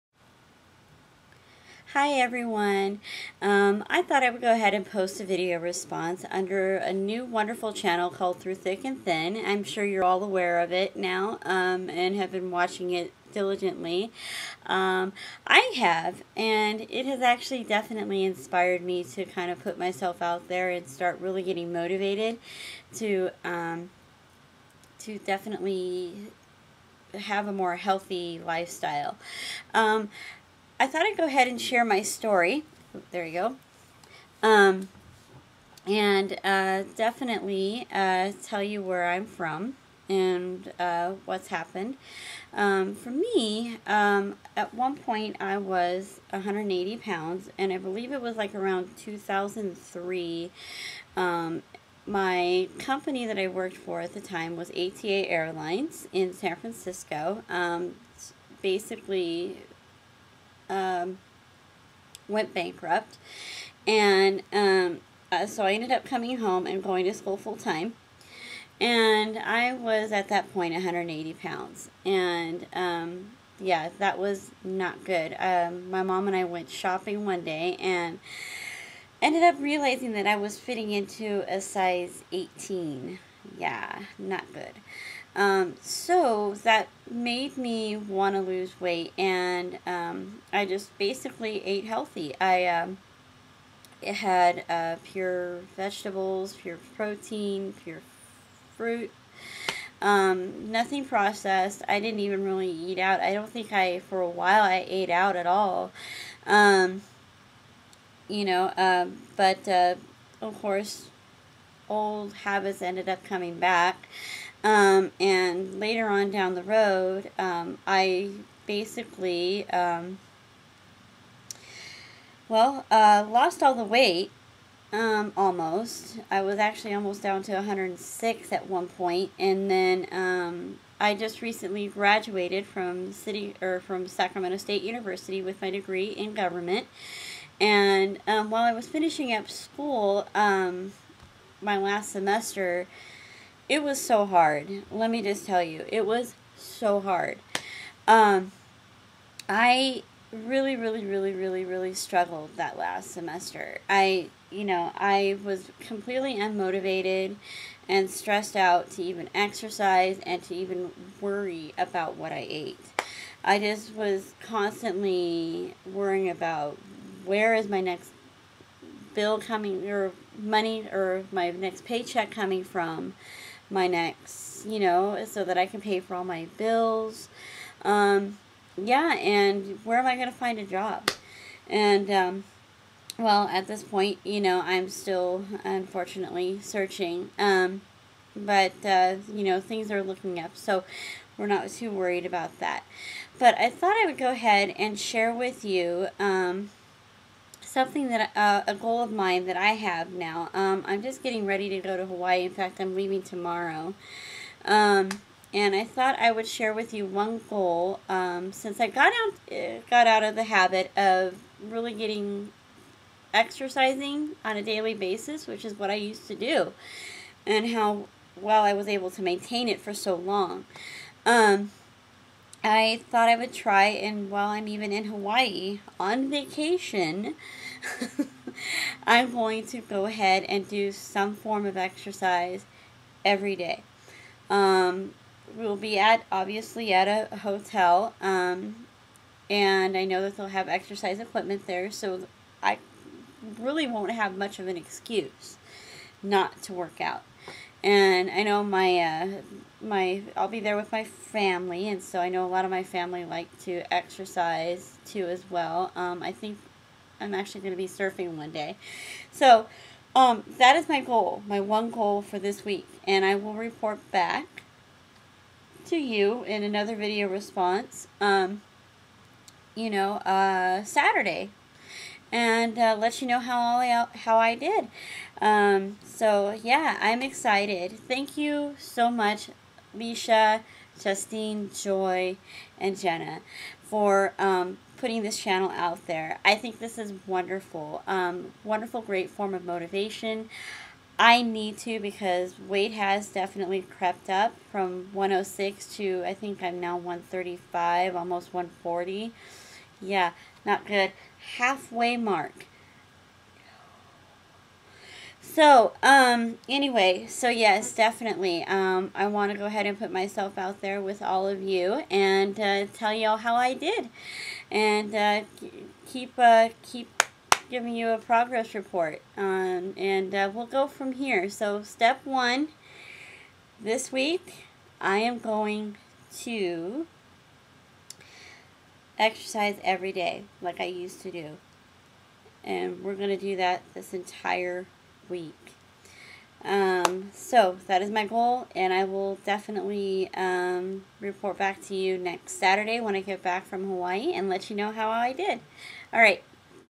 My Vlog-ThruThickandThin
Hey all!!! I dunno about you but I am motivated to get myself back on track. My goal this week is to workout everyday while I am in Hawaii. Wish me luck.